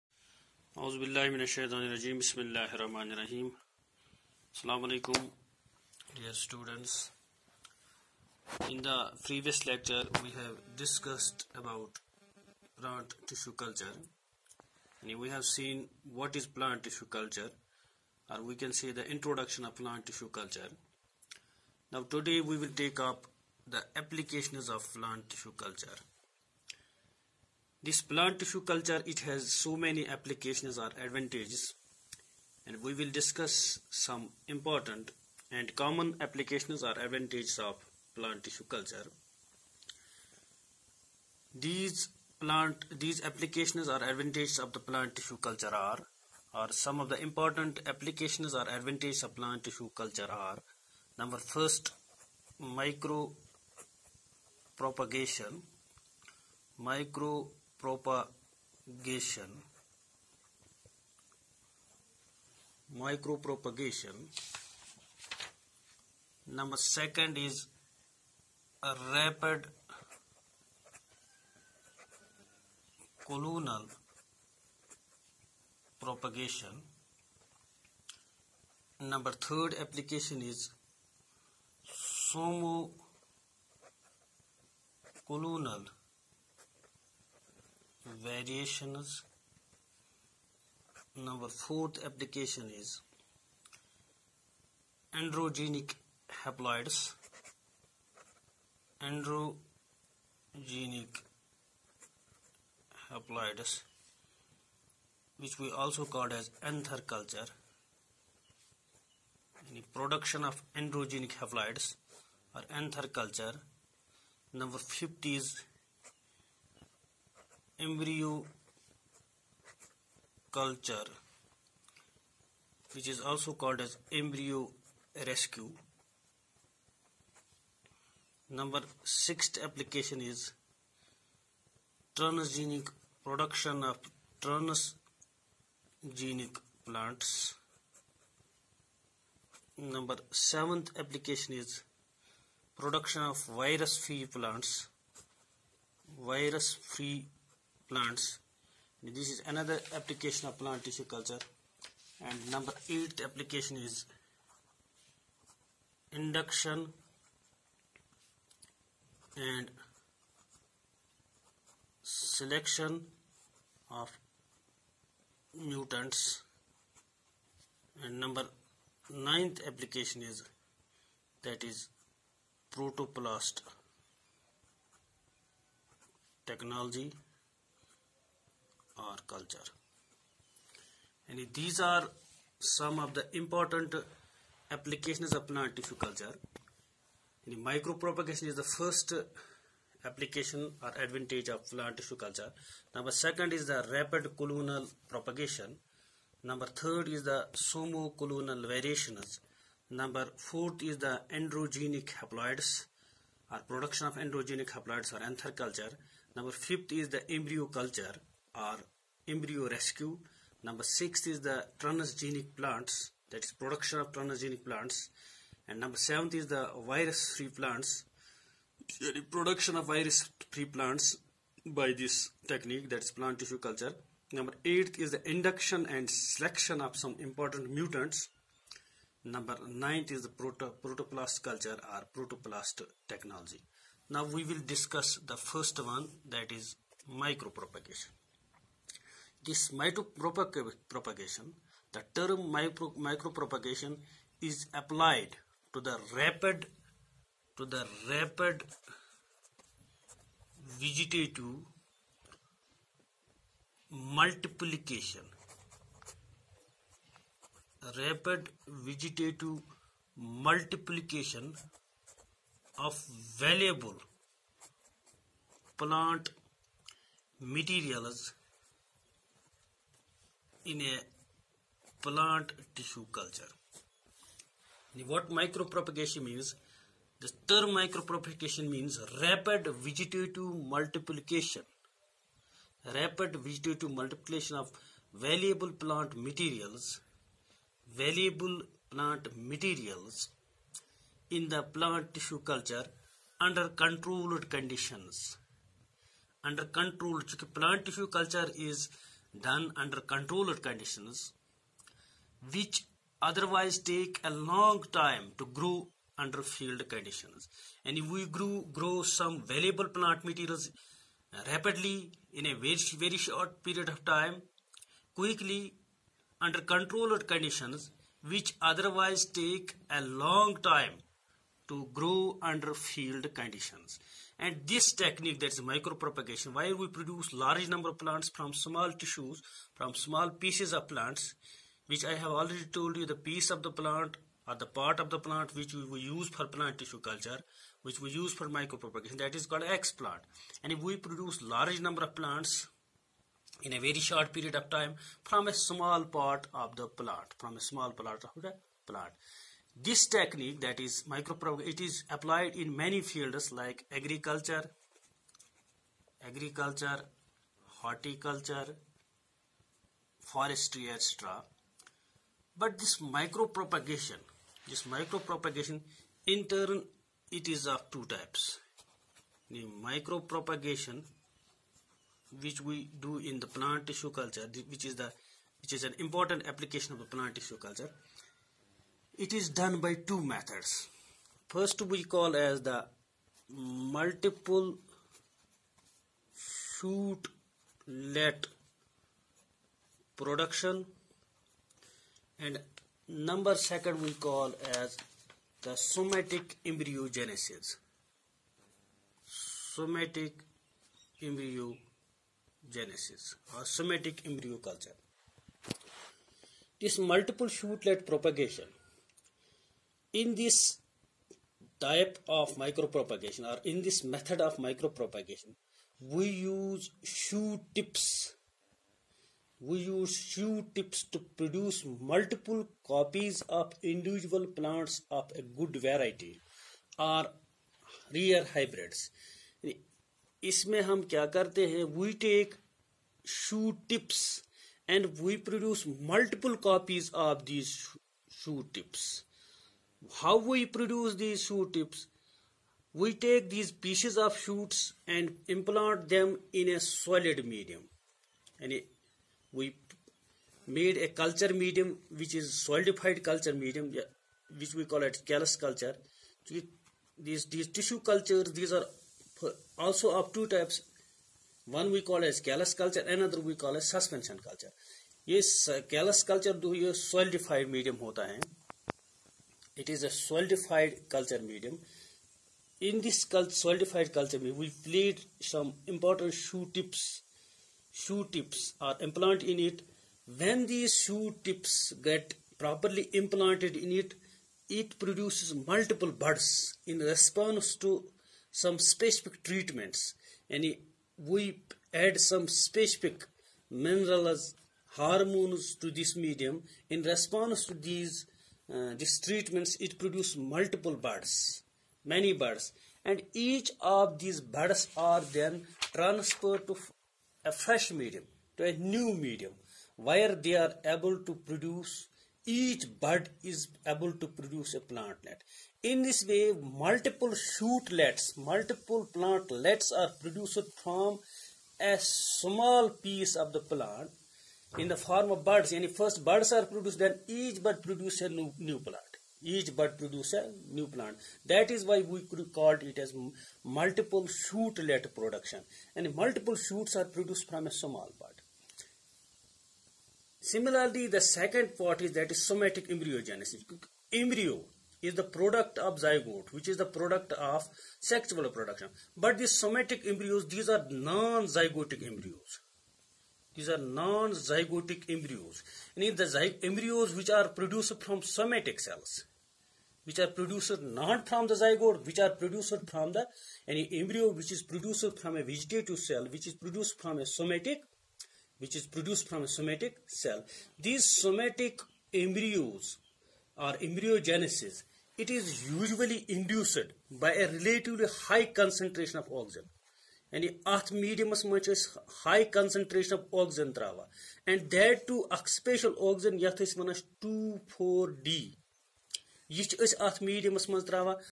Applications of plant tissue culture part-1
Class-12th Botany by Manzoor Ahmad Sheikh lecturer Botany Ghss langate